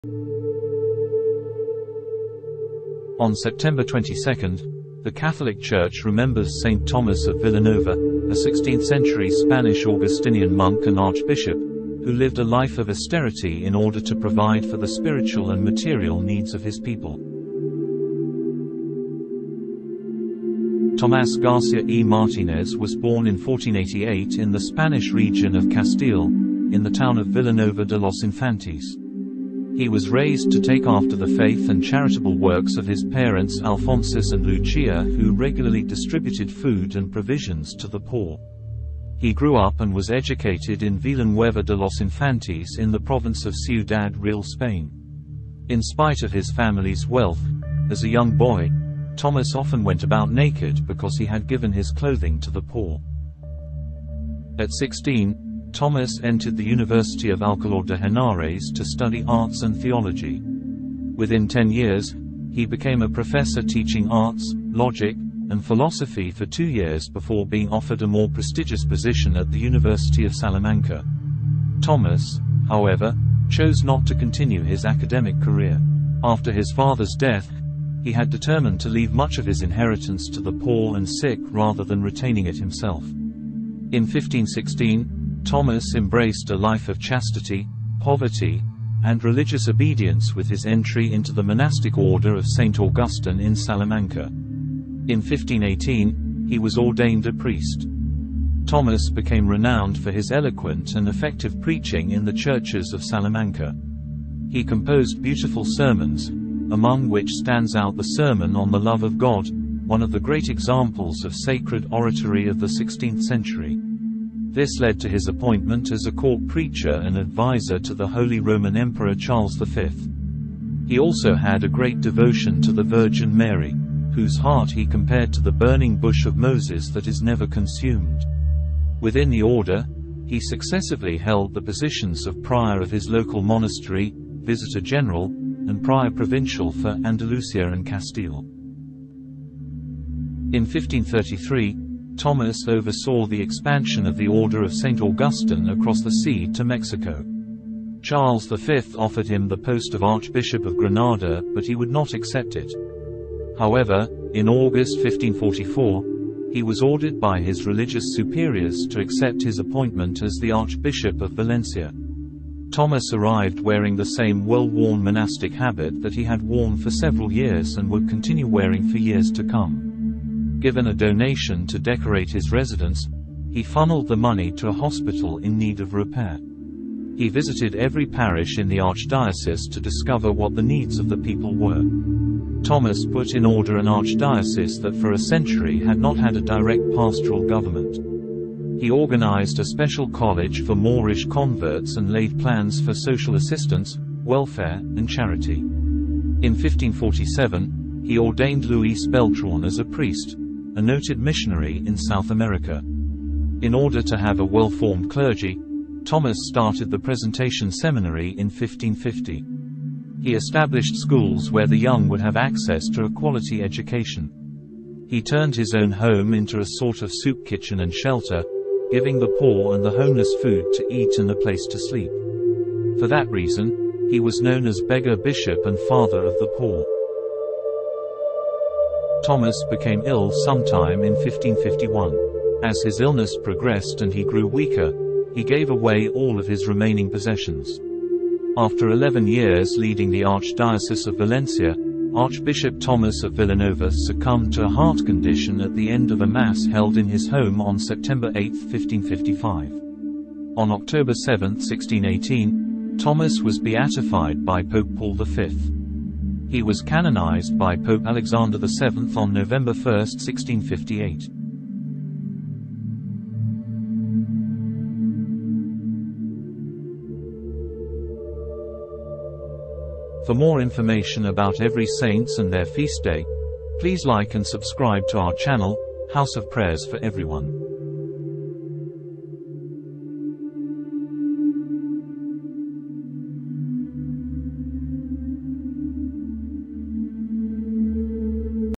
Saint of the Day - St. Thomas of Villanova (September 22nd)
On Sept. 22, the Catholic Church remembers Saint Thomas of Villanova, a 16th-century Spanish Augustinian monk and archbishop who lived a life of austerity in order to provide for the spiritual and material needs of his people.

Early Life

Tomás García y Martínez was born in 1488 in the Spanish region of Castile, in the town of Villanova de los Infantes. He was raised to take after the faith and charitable works of his parents Alphonsus and Lucia who regularly distributed food and provisions to the poor

He grew up and was educated in Villanueva de los Infantes, in the Province of Ciudad Real, Spain. In spite of his family's wealth, as a young boy, Thomas often went about naked because he had given his clothing to the poor.

Entering Priesthood

At 16, Thomas entered the University of Alcalá de Henares to study Arts and Theology. Within 10 years, he became a professor teaching arts, logic, and philosophy for two years before being offered a more prestigious position at the University of Salamanca.

Thomas, however, chose not to continue his academic career. After his father’s death, he had determined to leave much of his inheritance to the poor and sick rather than retaining it himself.

In 1516, Thomas embraced a life of chastity, poverty, and religious obedience with his entry into the monastic Order of St. Augustine in Salamanca. In 1518, he was ordained a priest.

Thomas, The Reformer

Thomas became renowned for his eloquent and effective preaching in the churches of Salamanca. He composed beautiful sermons, among which stands out the Sermon on the Love of God, one of the great examples of sacred oratory of the 16th century. This led to his appointment as a court preacher and adviser to the Holy Roman Emperor Charles V.

He also had a great devotion to the Virgin Mary, whose heart he compared to the burning bush of Moses that is never consumed. Within the Order, he successively held the positions of prior of his local monastery, Visitor General, and Prior Provincial for Andalusia and Castile.

Archbishop of Valencia

In 1533, Thomas oversaw the expansion of the Order of St. Augustine across the sea to Mexico. Charles V offered him the post of Archbishop of Granada but he would not accept it. However, in August 1544, he was ordered by his religious superiors to accept his appointment as the Archbishop of Valencia.

Thomas arrived wearing the same well-worn monastic habit that he had worn for several years and would continue wearing for years to come. Given a donation to decorate his residence, he funneled the money to a hospital in need of repair.

He visited every parish in the Archdiocese to discover what the needs of the people were. Thomas put in order an archdiocese that for a century had not had a direct pastoral government. He organized a special college for Moorish converts and laid plans for social assistance, welfare, and charity.

In 1547, he ordained Luis Beltrán as a priest - a noted missionary in South America.

In order to have a well-formed clergy, Thomas started the Presentation Seminary in 1550. He established schools where the young would have access to a quality education. He turned his own home into a sort of soup kitchen and shelter, giving the poor and the homeless food to eat and a place to sleep. For that reason, he was known as Beggar Bishop and Father of the Poor.

Death and Veneration

Thomas became ill sometime in 1551. As his illness progressed and he grew weaker, he gave away all of his remaining possessions. After 11 years leading the Archdiocese of Valencia, Archbishop Thomas of Villanova succumbed to a heart condition at the end of a Mass held in his home on Sept. 8, 1555.

On October 7, 1618, Thomas was beatified by Pope Paul V. He was canonized by Pope Alexander VII on November 1, 1658.

Images attributed to the following:
- By Grabado de Luis Fernández Noseret por dibujo de José Maea - Libro «Retratos de Españoles ilustres» publicado por la Real Imprenta de Madrid. Según la información brindada en el prólogo, las imágenes contenidas y los textos fueron realizados por distintos autores., Public Domain, Wikimedia Commons
- By Bartolomé Esteban Murillo Public Domain, Wikimedia Commons